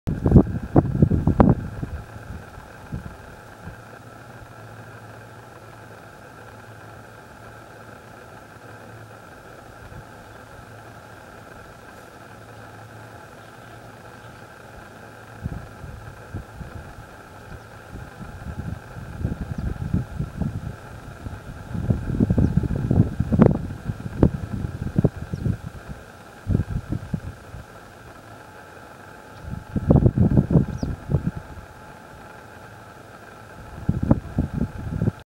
Torenvalken met Spreeuwen
Er zit een jonge torenvalk rustig in een boom als hij ineens een spervuur aan spreeuwen om zich heen in de boom ziet landen.
Een andere torenvalk van het nest (broer of zus) komt vervolgens te hulp en schrikt de spreeuwen met twee gerichte scheervluchten weg.
Ze blijven samen in de boom over en gaan lekker zitten poetsen.
Opgenomen met min Canon 7D in het Limburgse heuvel landschap tijdens mijn vakantie.
Gr. Henk Schuurman